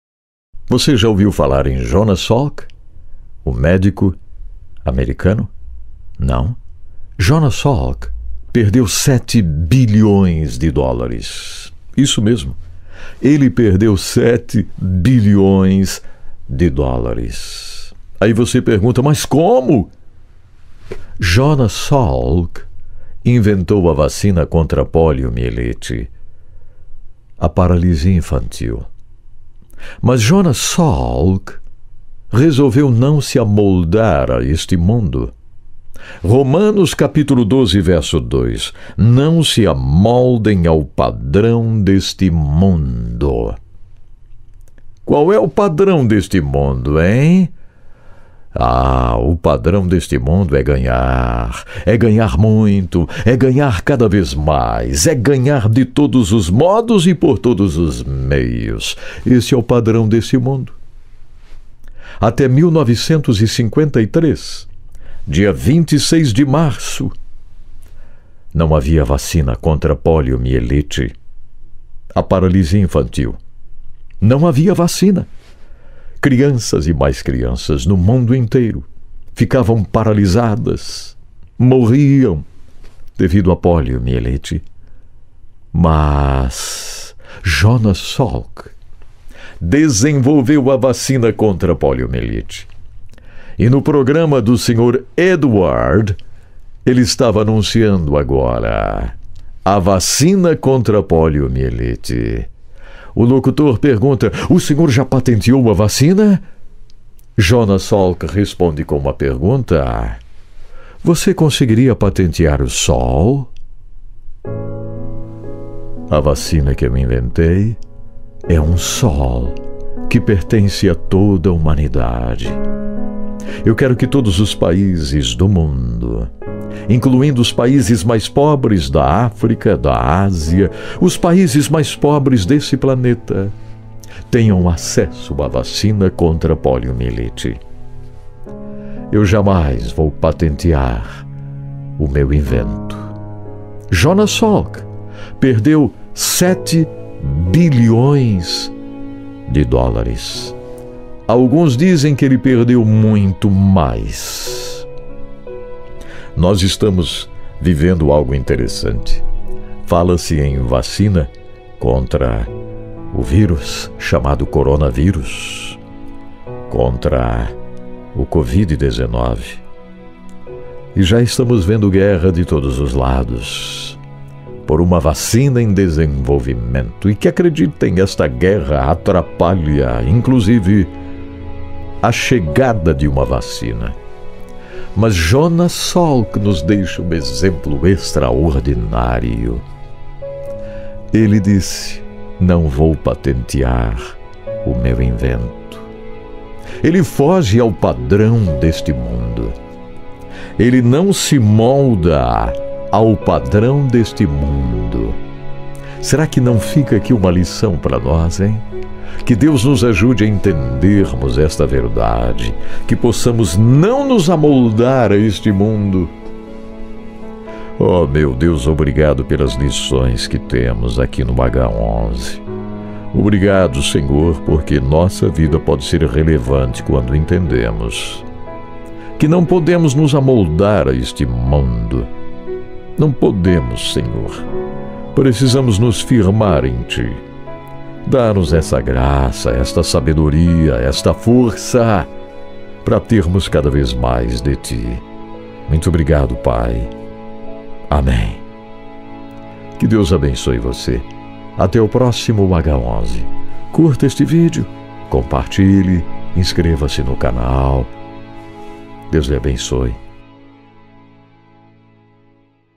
Jonas Salk | Pr. Edson Bruno
O H11 de hoje apresenta o grande exemplo de Jonas Salk. Ele não se amoldou aos padrões deste mundo. Vale a pena você assistir na íntegra mais. Compartilhe, curta e comente com sabedoria e inspiração.
✔ 👇 Siga o Encontro Com a Palavra no Instagram
✔ 👇 E cadastre-se gratuitamente em nossos cursos